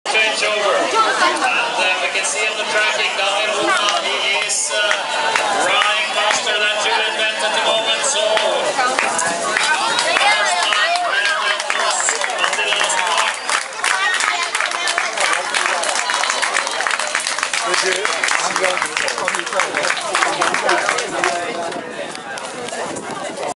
orienteering- 3rd leg Jan SEDIVY (CZE) pass spectator control
20090719 world games 2009 kaohsiung orienteering relay Chengcing Lake